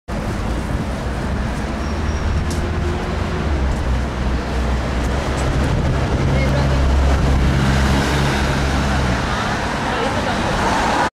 5th Feb 2012 Edinburgh Part 24
The place where J.K Rowling wrote Harry Potter